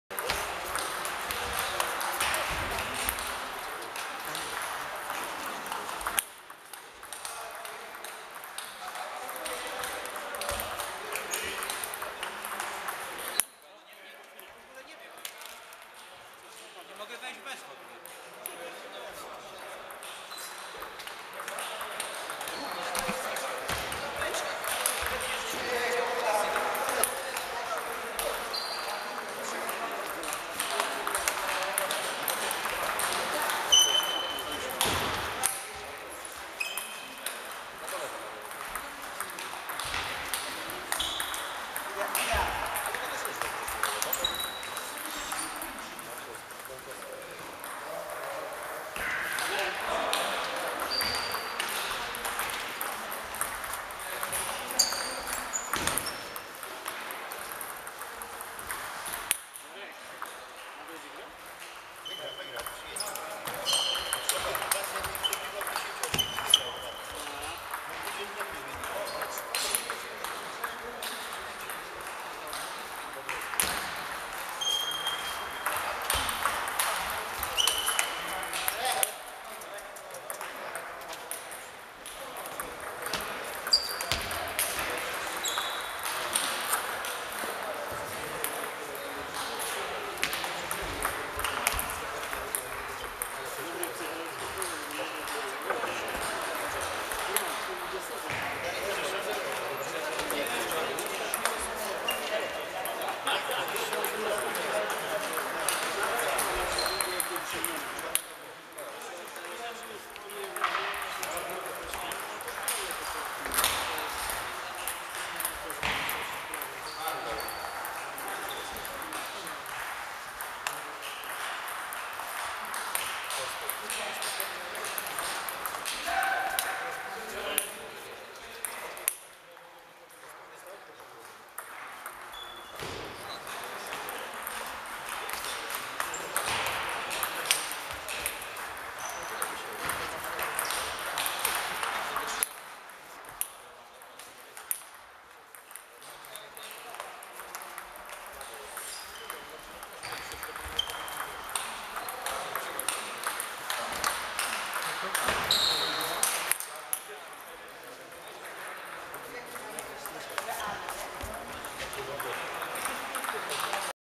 Michał Markowski-Arkadiusz Zieliński set.2.MOV
5 Turniej XI cyklu Grand Prix Amatorów o Puchar Dyrektora OSiR Gminy Warszawa Targówek m.st. Warszawy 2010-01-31
turniej open